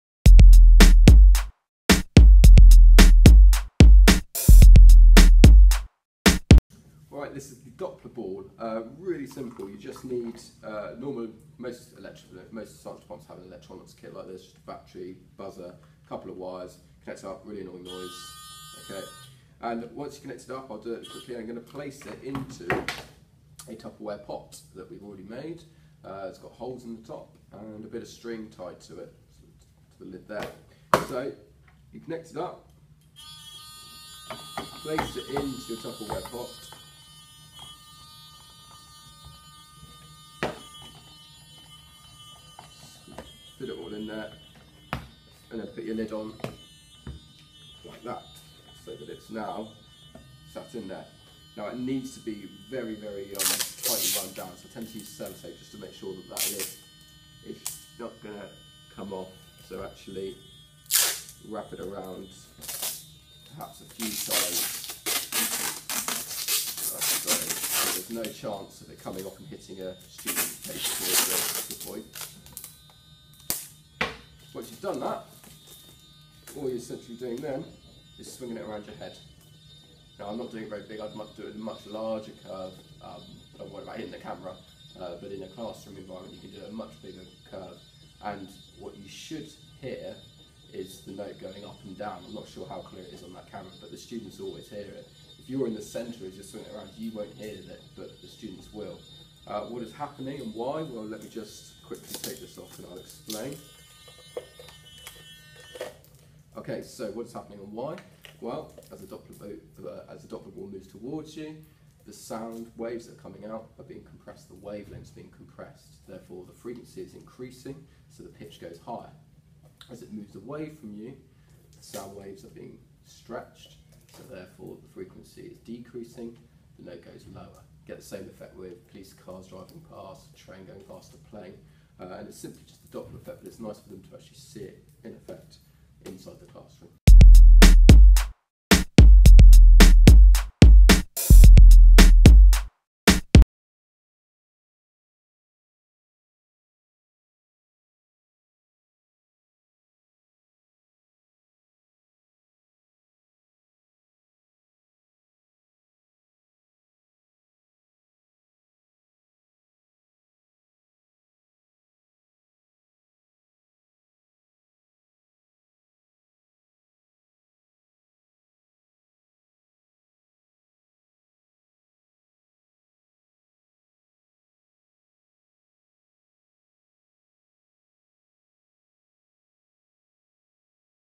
Physics Teacher Toolkit 'Doppler Ball Demonstration'
Great demonstration to use in the Science classroom when teaching the Doppler Effect. Key words: Doppler effect, frequency, sound, pitch, red-shift, Big Bang